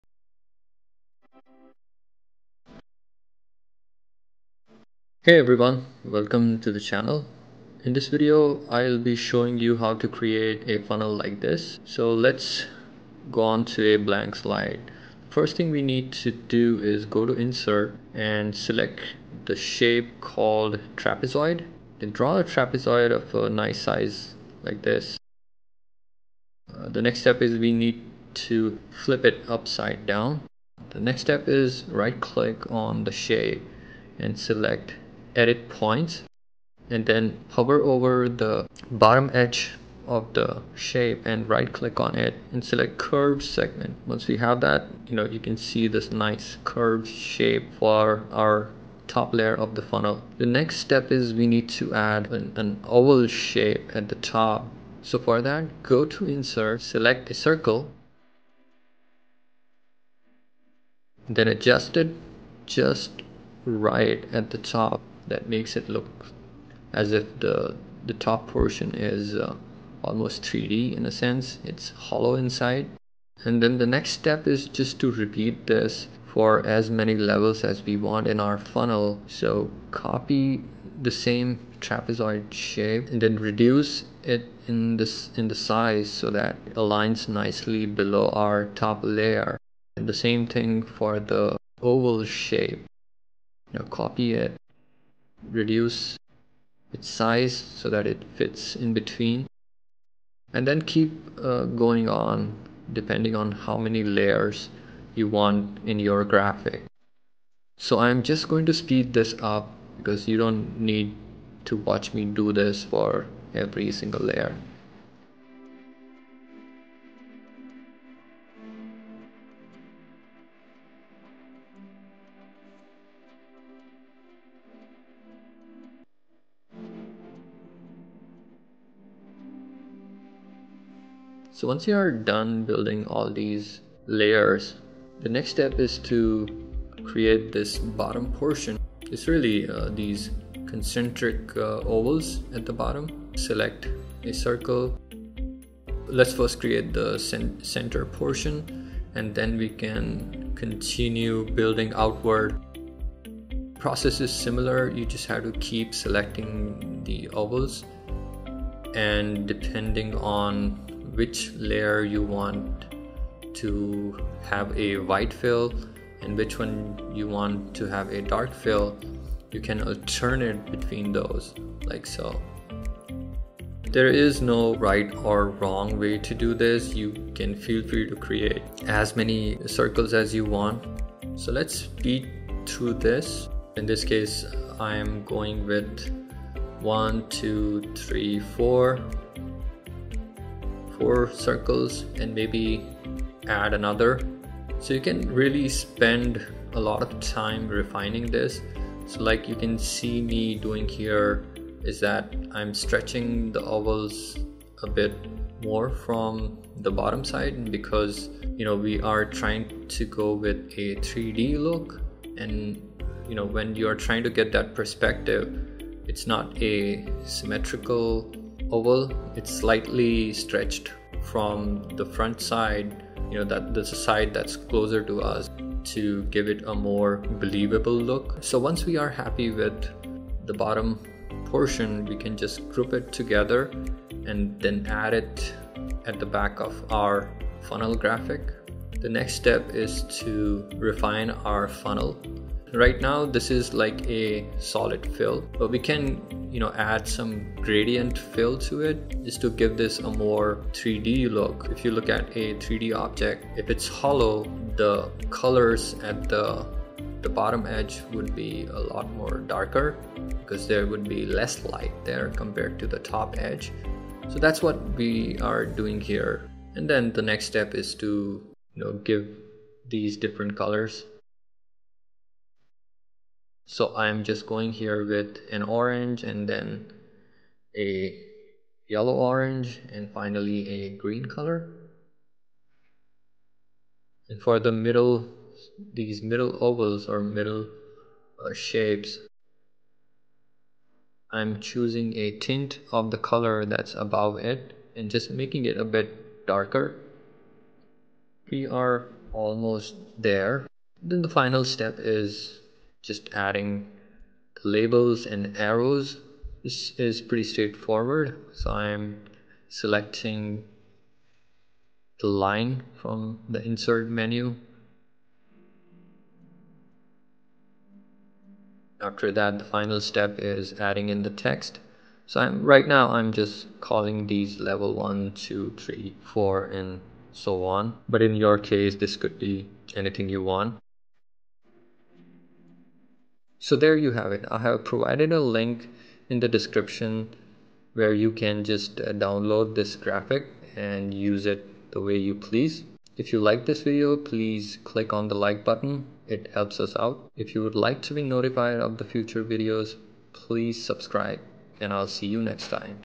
How to create a Funnel diagram in PowerPoint
In this video, I demonstrate how to create a funnel graphic in PowerPoint.

🙋Consider becoming a member for more perks and to help this channel grow.